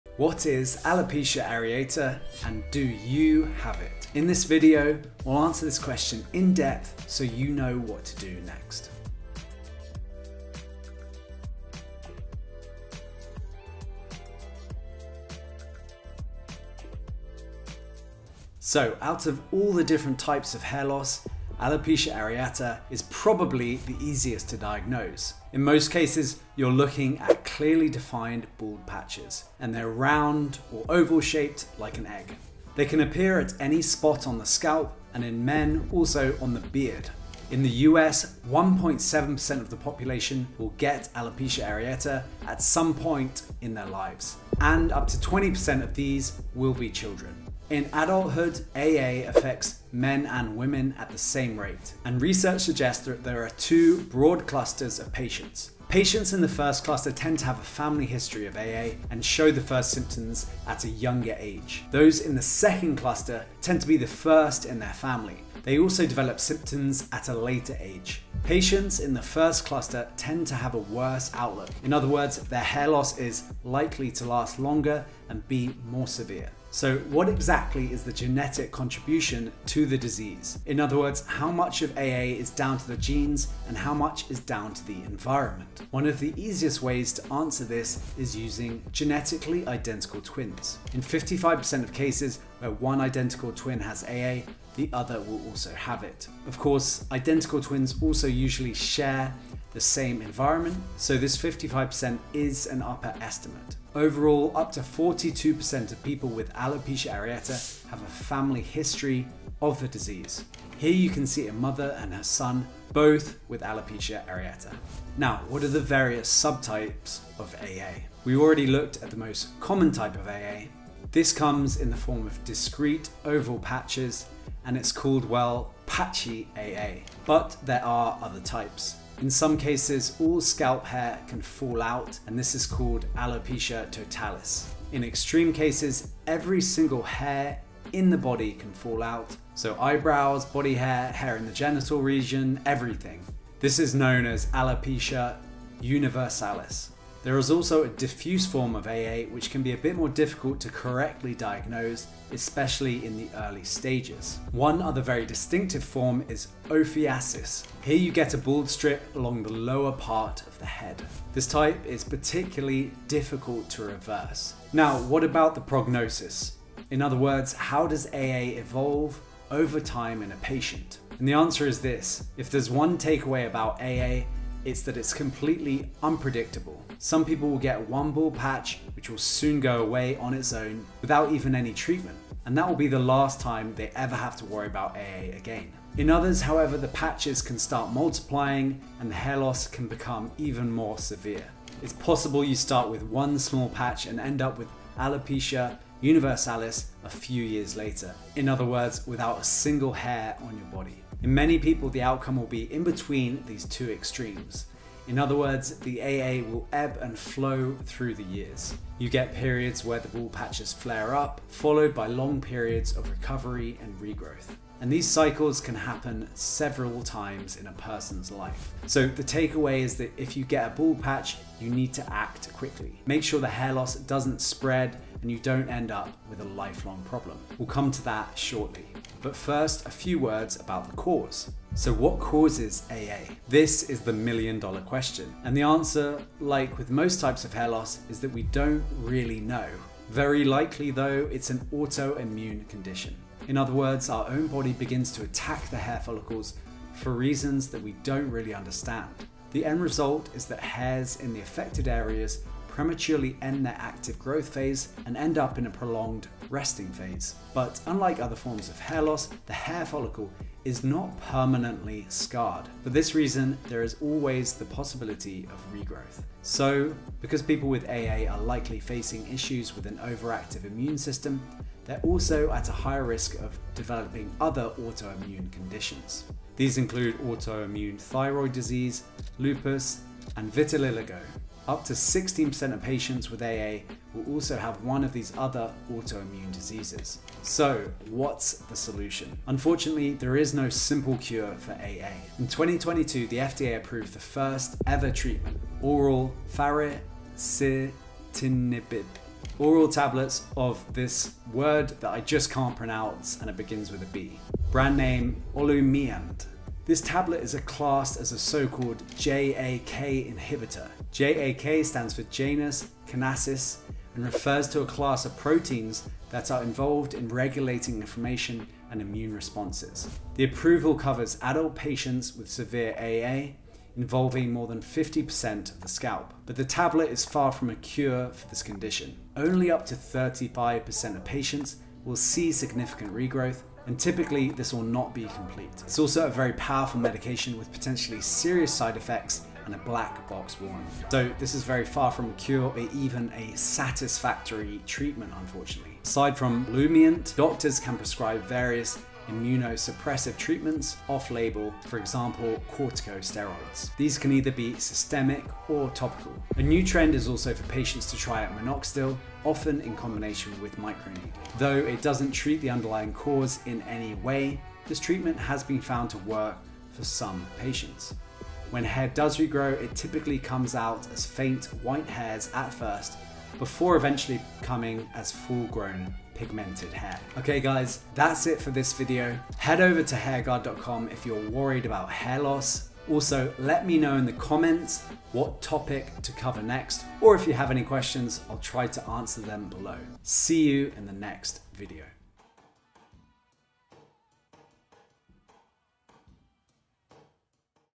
Alopecia Areata - 2024 UPDATE
What is Alopecia Arcata and is there a treatment for it?